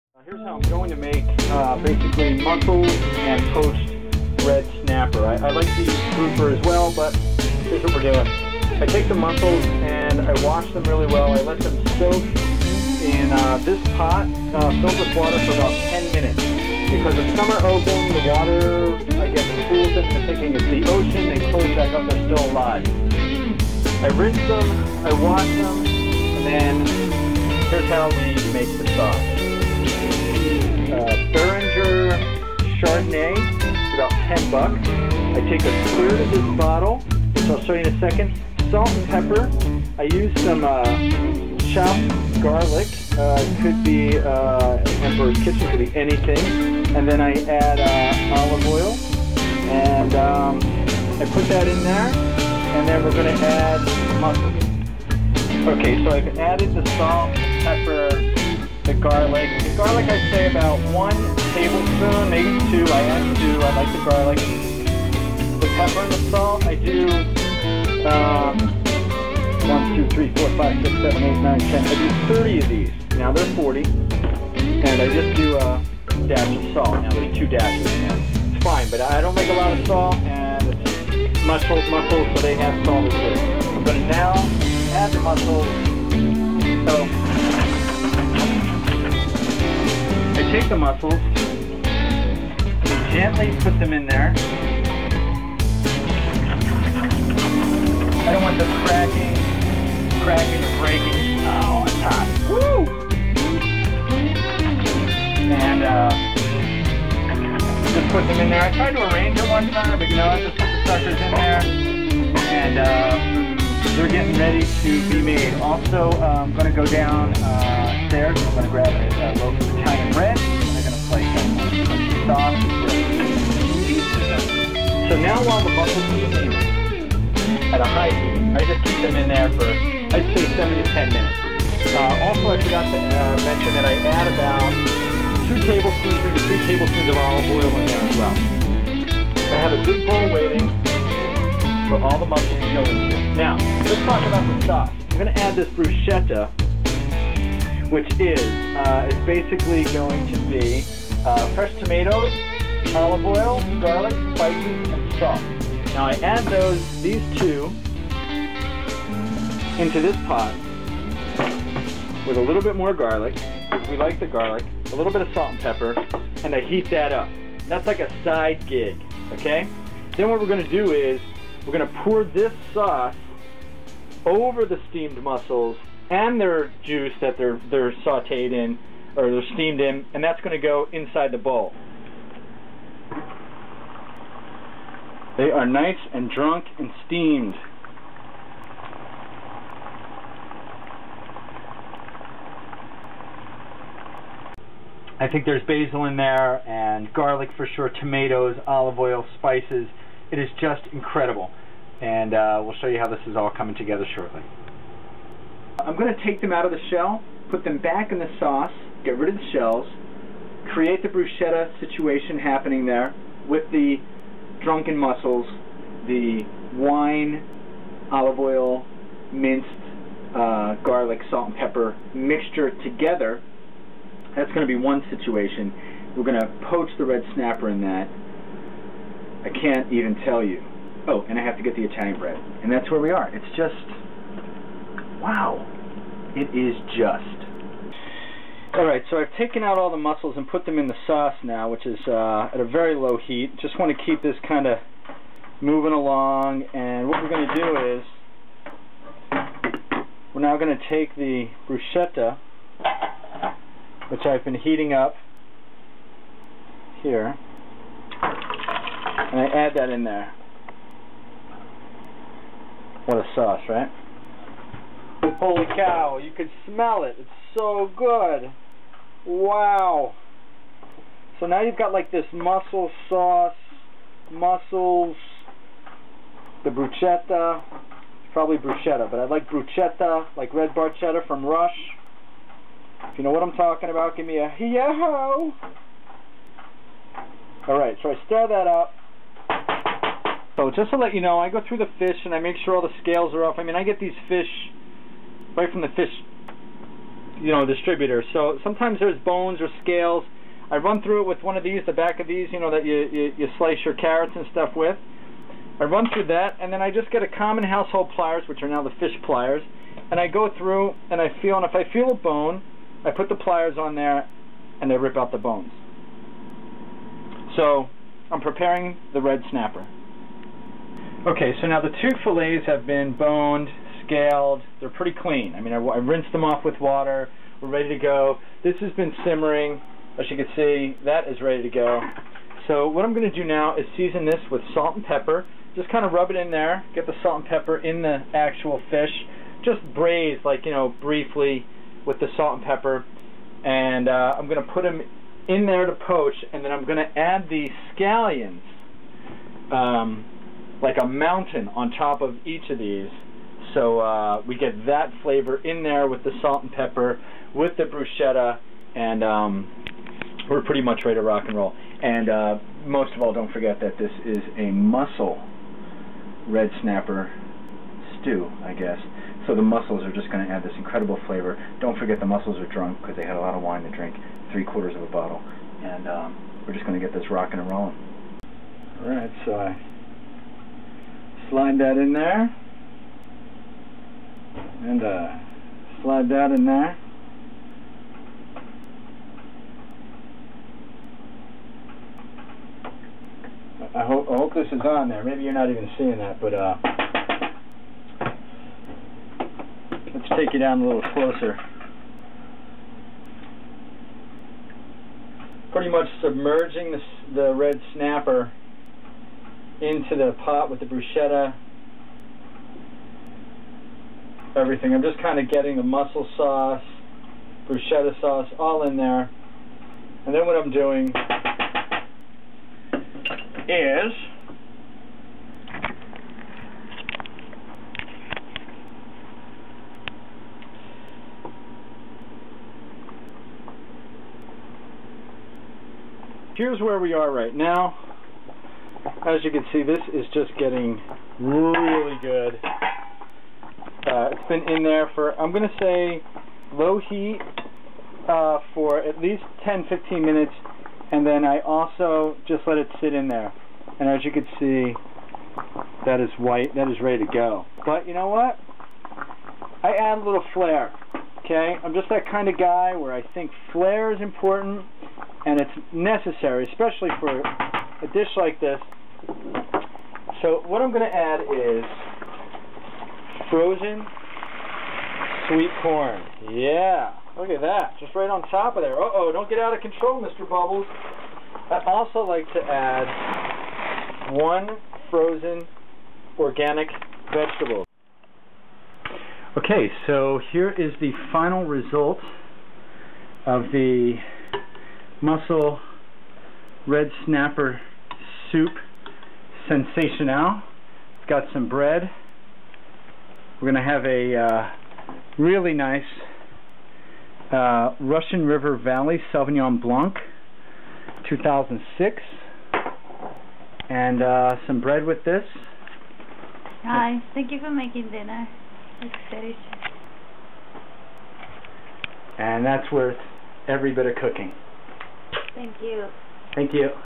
Robert's Mussel and Red Snapper Soup
Red Snapper Filets, 3 pounds of Mussels, two jars of Bruschetta, salt, pepper, garlic, 3/4 bottle of white wine, poach the fish. Eat with loaf of Italian bread and a bottle or two of White Wine~~Enjoy!